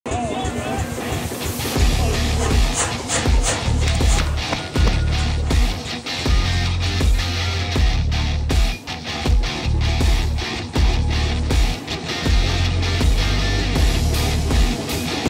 Our power hammer turns Terrapin’s ‘Hopsecuter IPA’ into DUST!
Our power hammer turns Terrapin’s ‘Hopsecuter IPA’ into DUST! What should we smash next? And… what have you been sippin’ lately?